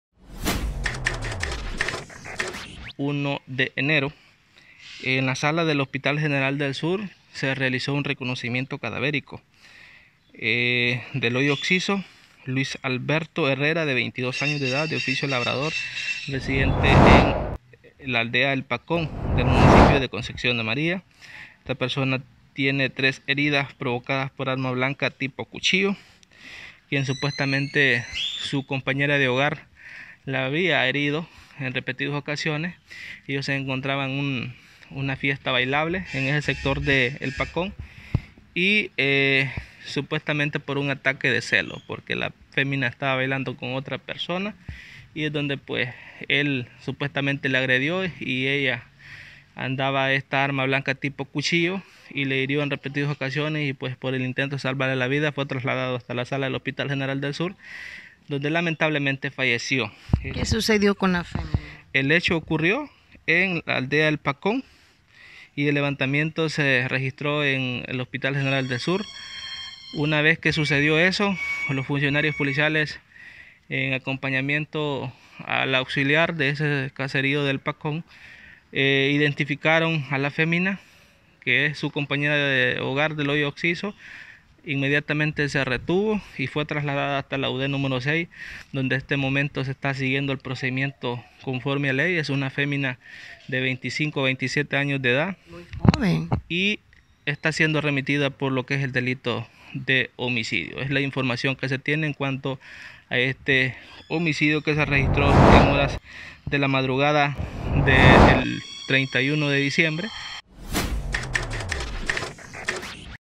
MUJER MATA DE TRES PUÑALADAS A SU MARIDO DURANTE UNA DISCUSIÓN EN CONCEPCIÓN DE MARÍA
Lametrohn.com